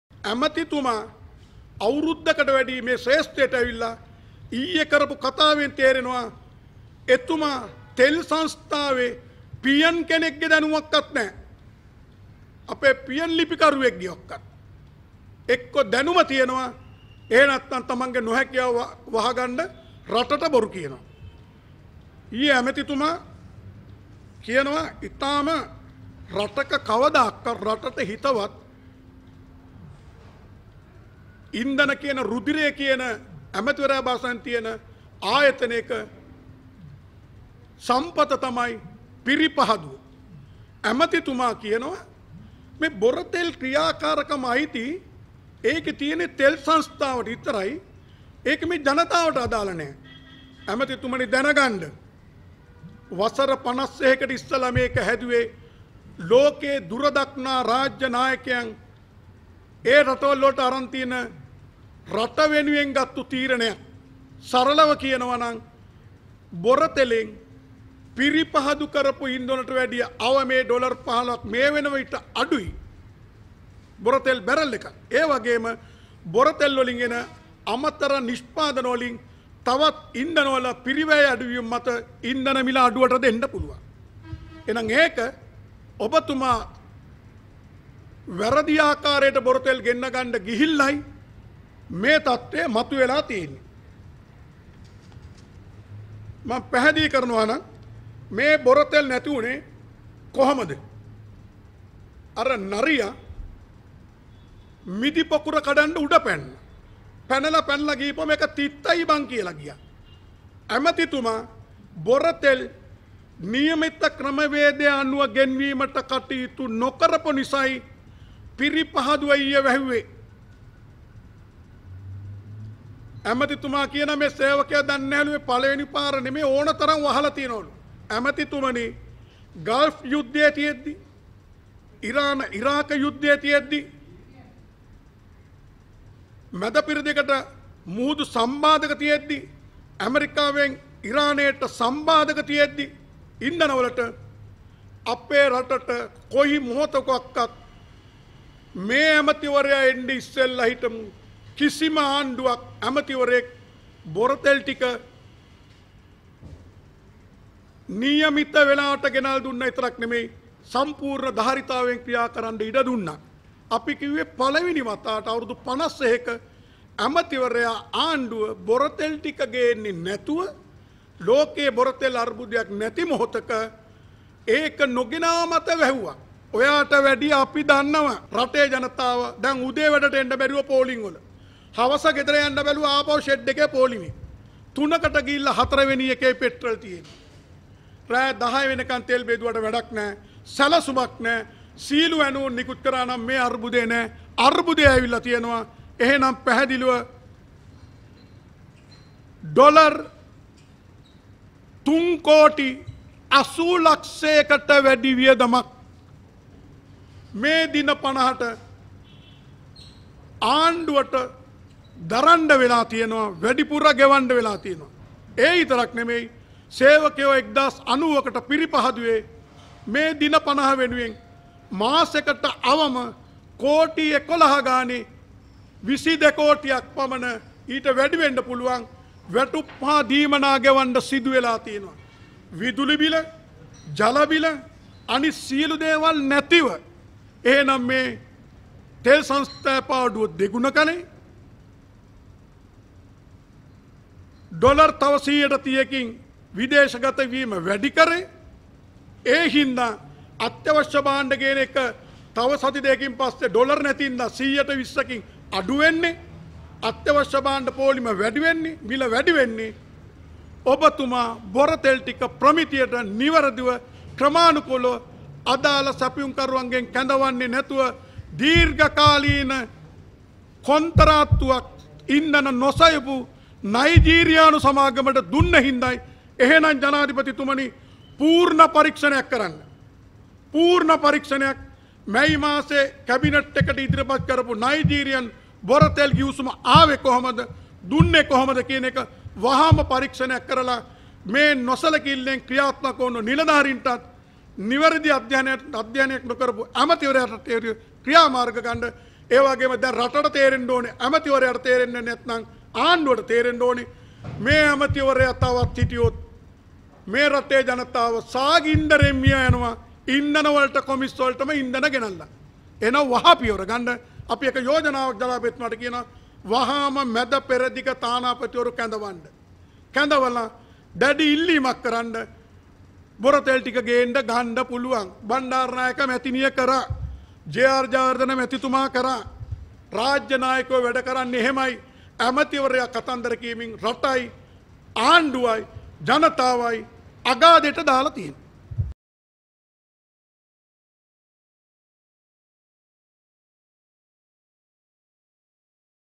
උදයගම්මන්පිලට පියන් කෙනෙක්ගේ දැනුමක්වත් නෑ ඔයාට වැඩිය අපි දන්නවා -ආනන්ද පාලිත | Breaking.lk
උදයගම්මන්පිලට පියන් කෙනෙක්ගේ දැනුමක්වත් නෑ ඔයාට වැඩිය අපි දන්නවා  -ආනන්ද පාලිත